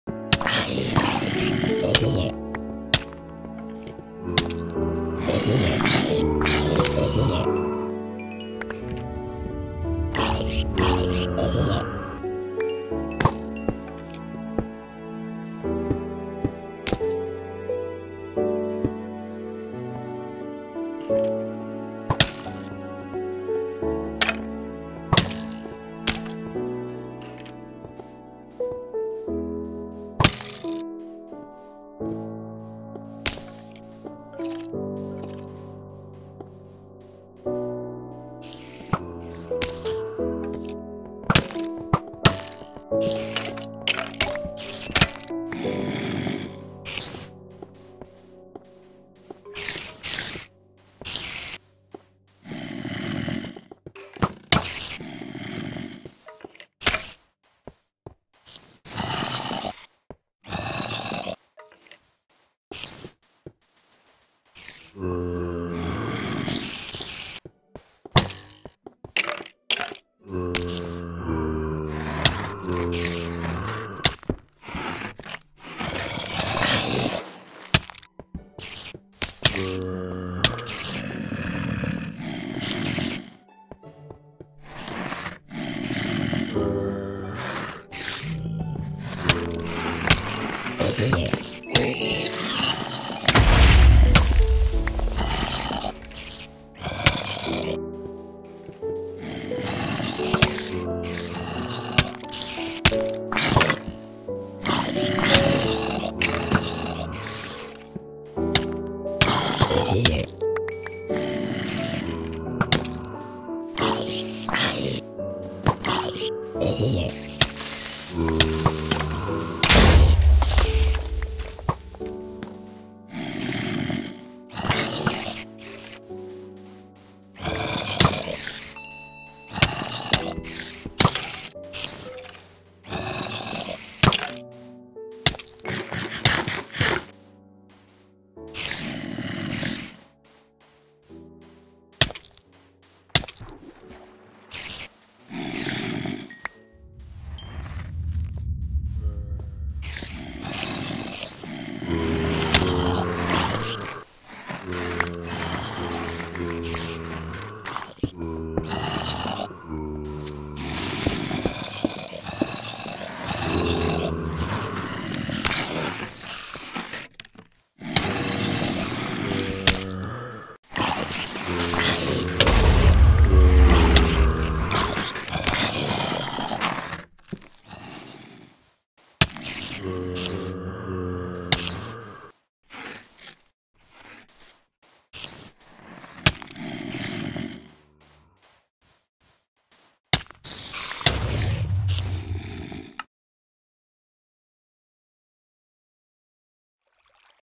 Inv mod p2
Invasion mod part 2, This mod is where you create a nexus and face the horde of mobs that try to destroy it. Please post something if you want me to show you how to install/create the ingrediants. TO VIEW IN HD PLEASE PUT &fmt=37  AT THE END OF THE ADRESS LINK